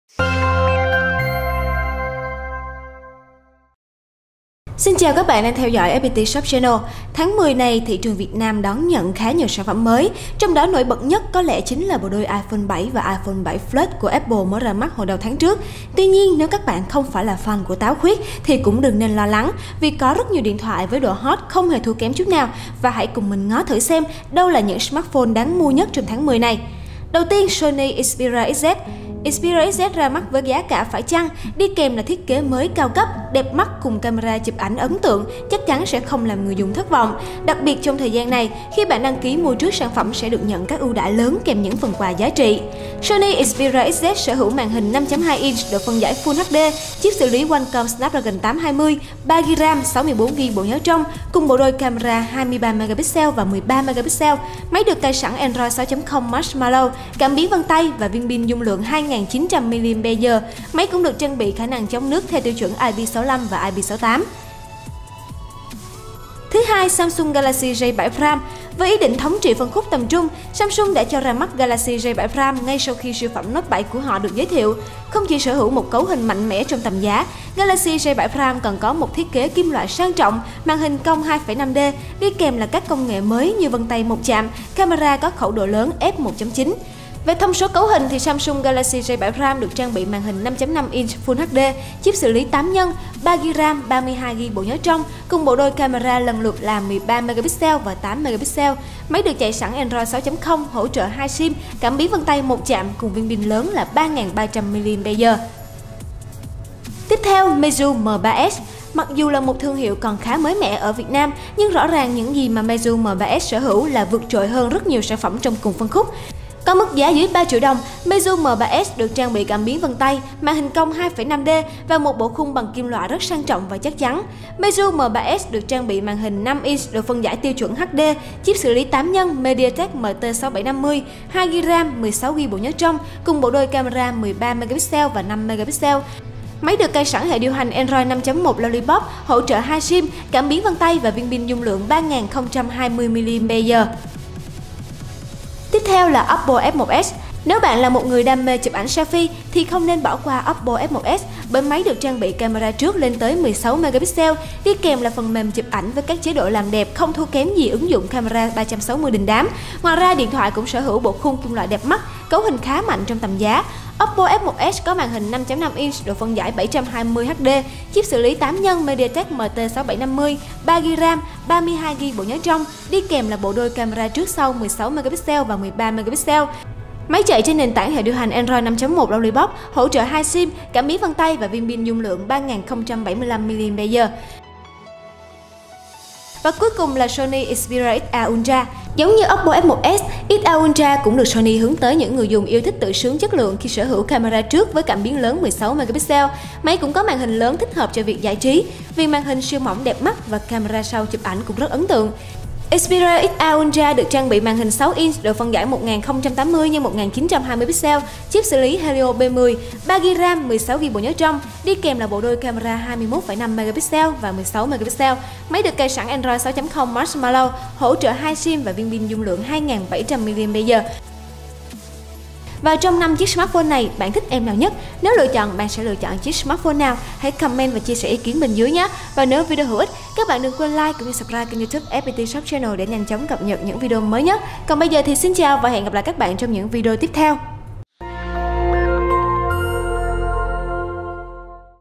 FPT Shop - Top 5 smartphone đáng sở hữu trong tháng 10
Top 5 smartphone đáng sở hữu trong tháng 10

- sony xperia xa ultra
- meizu m3s
- oppo f1s
- galaxy j7 prime
- sony xperia xz